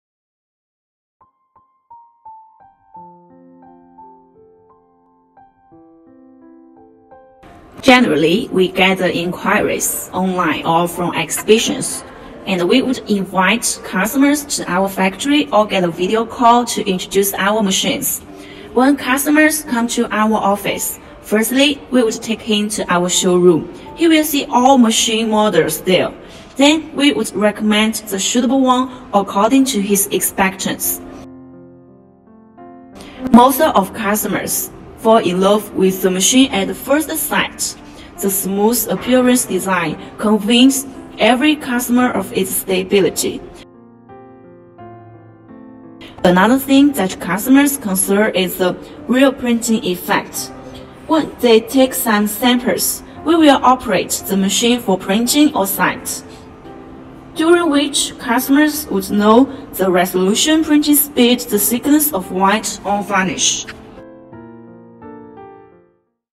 The process that customers cooperate with Nocai (Part 1)
1.Generally we get the inquiries online or from exhibitions .And we
 would invite customers to our factory or  get a video call to
introduce our machines.

2.When customers come to our office , firstly , we would take him to
our showroom. He will see all machine models there. Next, we would
recommend the suitable one according to his expectance.

3.Most of customers fall in love with the machine at the first sight ,
the smooth appearance design  convince every customer of its stability.

4.Another thing that customers concern is the real printing effect .
When they take some samples we will operate the machine for printing on
 site, during which ,customers would know the resolution ,printing
speed ,the thickness of white or varnish etc.